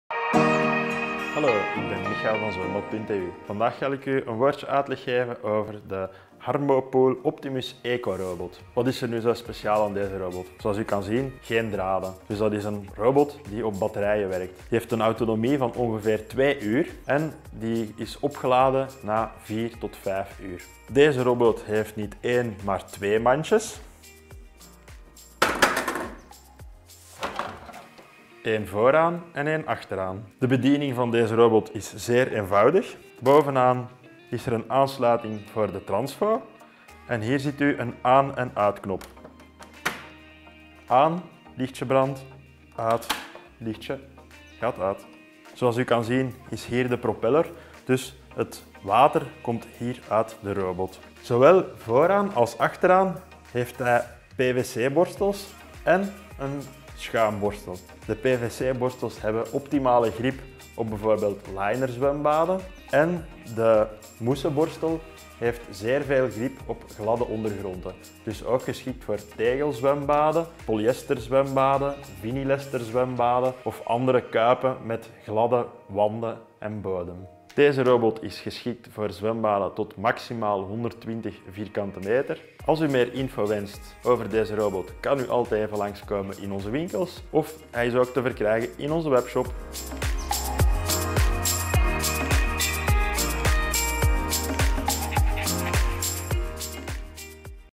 Harmo Pool Optimus Eco zwembadrobot
In deze video presenteert Michaël van Zwembad.eu u de Harmo Pool Optimus Eco zwembadrobot. Meer informatie vindt u op